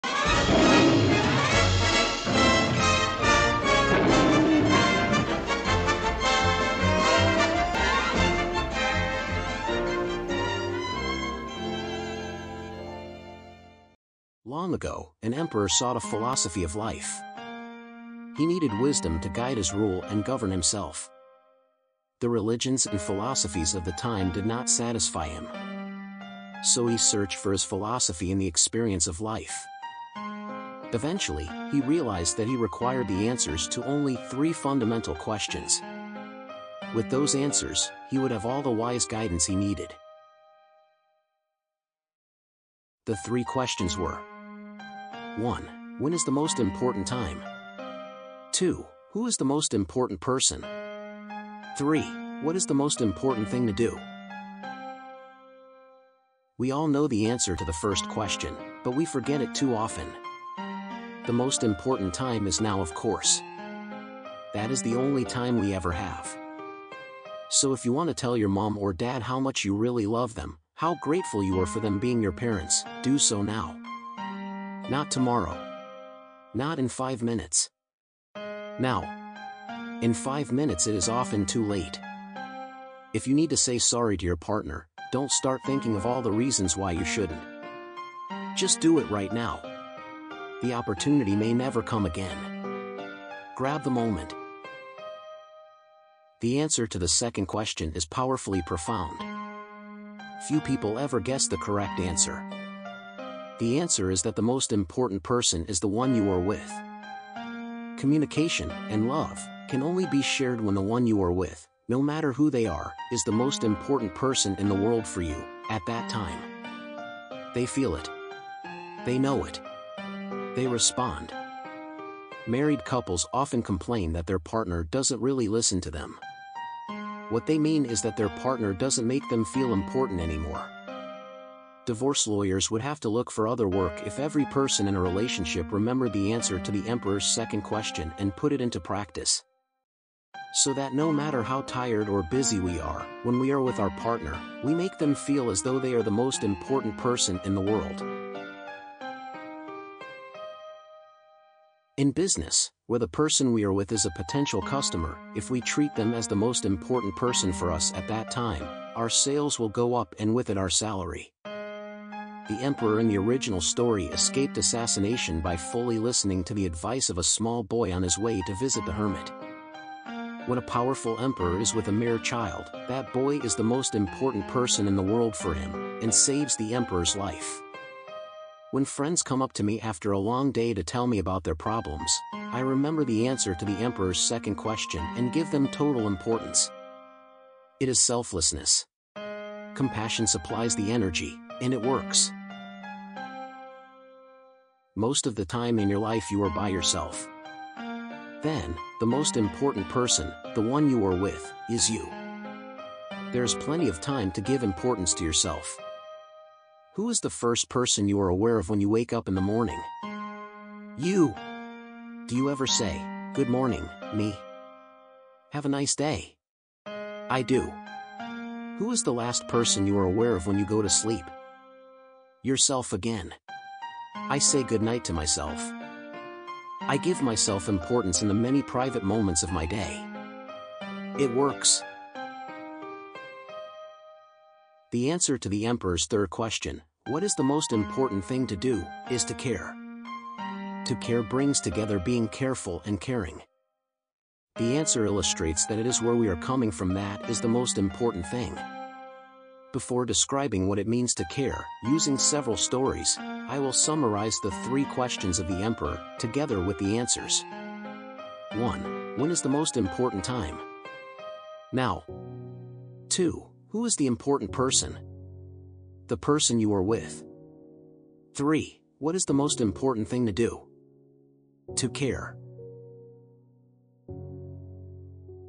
The Emperor’s three questions
The story actually taken from the Ajahn Brahm's retreat and his book entitled "Opening the Door of your Heart". The presentation probably is not good as the professional but hope you like it.
Have a nice day. Thanks